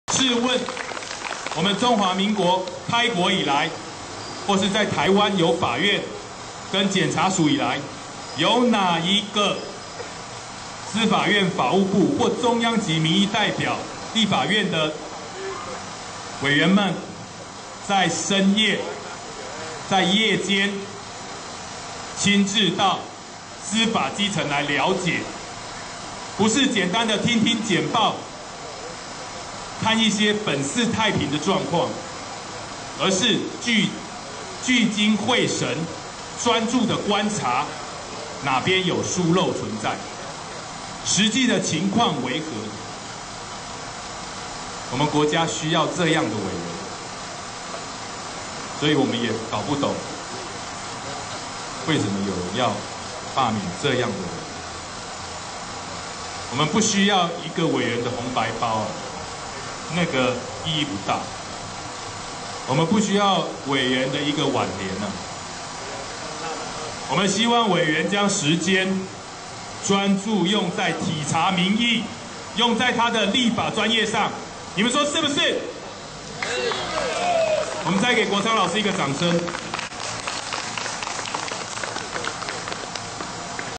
【罷免黃國昌的二十個理由！系列八】｜國會調查兵團 cic.tw
【煽動法警降低三十小時加班過勞！不罷免嗎？？】請強力分享！
【罷免黃國昌的二十個理由！系列八】
法警連續加班三十小時，竟然還抱怨加班費一小時６０太低～是過勞法警！
黃國昌也是～還跑到台中過夜要跟法警一起～
台灣的經濟已經夠不景氣了，工總都說過了 台灣勞工薪水要是調到27000，企業不是出走就是收起來不做了，老黃你竟然還要公務員帶頭爭取加班費提高～
你難道不怕政府也出走嗎？咦！ 您不罷免 黃國昌嗎？？

國會調查兵團FB粉專